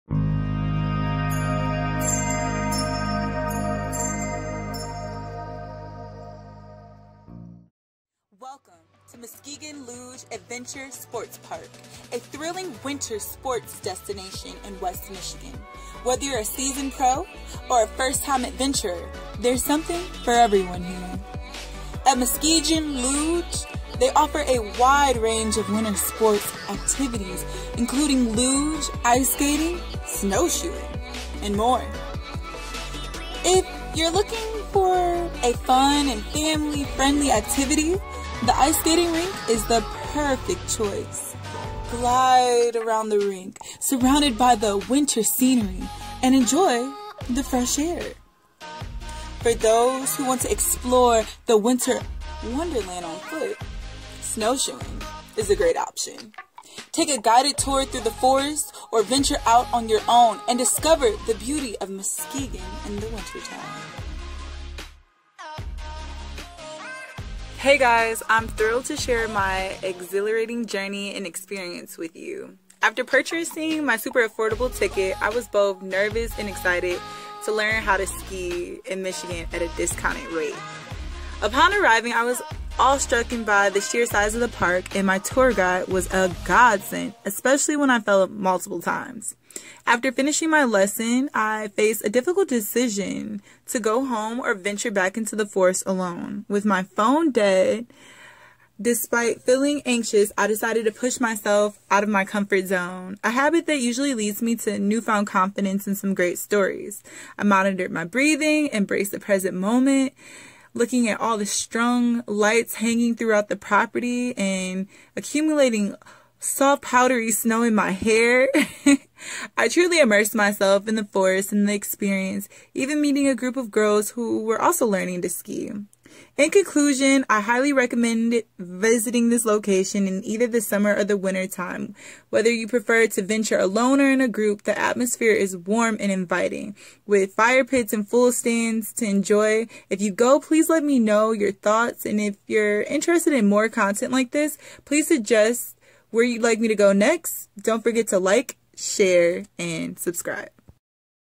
Muskegon, Michigan Adventure for Solo Travelers
Get Your Free Travel Guide!!!

Calling all solo travelers! Get ready to experience the thrill of a lifetime at Muskegon Luge. This hidden gem in Michigan offers a one-of-a-kind adventure that will ignite your sense of wonder. Join me as I dive into the exhilarating world of solo travel, exploring the state's most awe-inspiring landscapes while embracing the freedom and empowerment that comes with it.